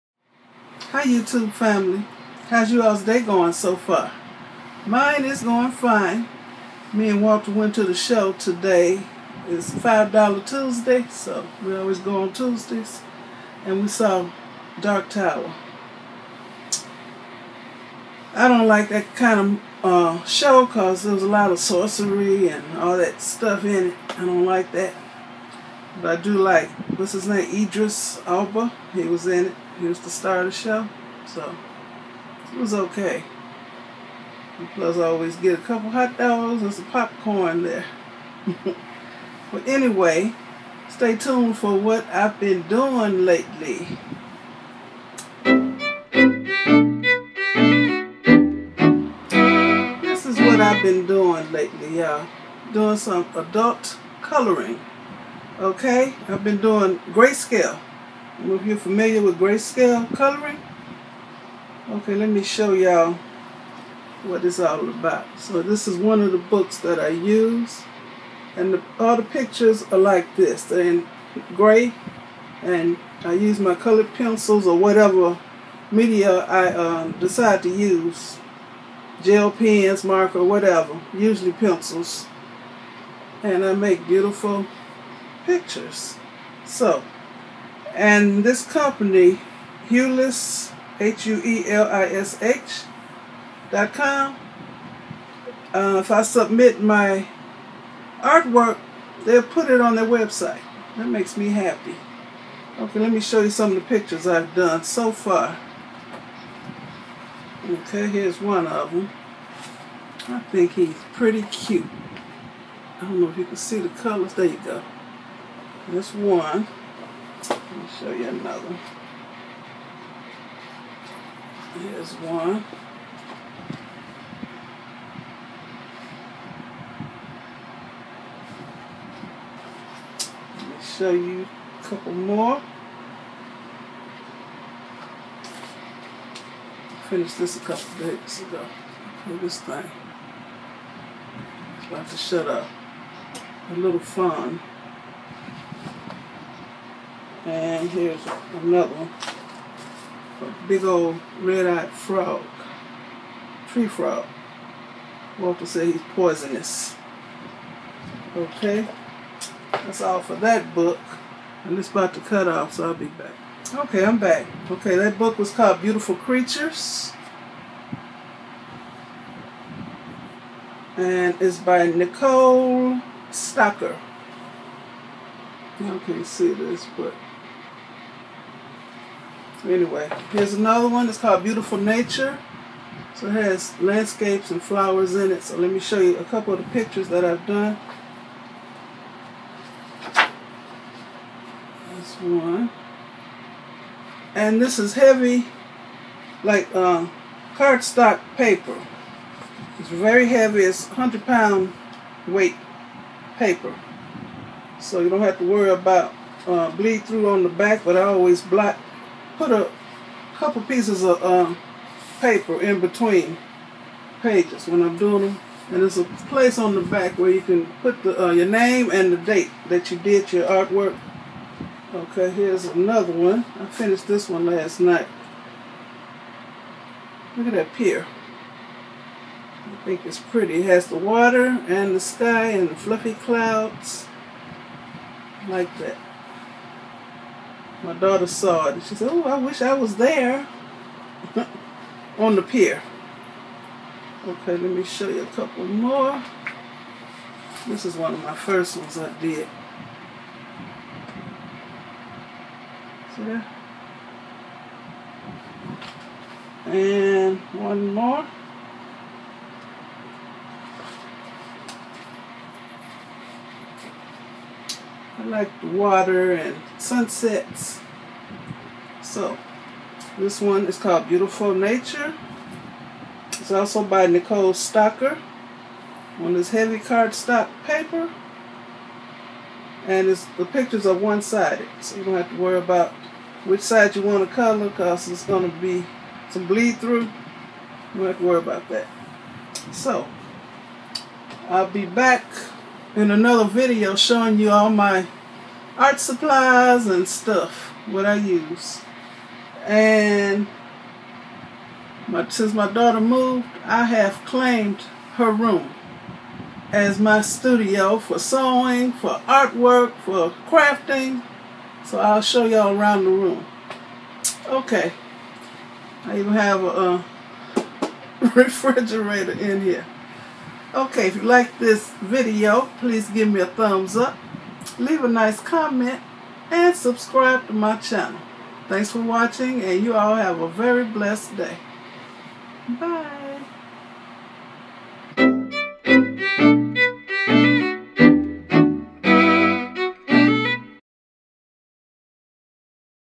#550 - GRAYSCALE Coloring/ What I've Been Up To!
I have joined the 'ADULT' coloring craze and it's VERY relaxing! I decided to do 'GRAYSCALE' mainly to learn more about shade and light to help me with my acrylic painting. I used Prisma Premier Colored Pencils. Coloring "GRAYSCALE' is nothing like your kids coloring books! Please comment & thumbs UP! 💞
I paid for EVERYTHING in this video!
BEAUTIFUL CREATURES
BEAUTIFUL NATURE
Send PRODUCTS for review to:
MzBrazell
P.O.Box 2153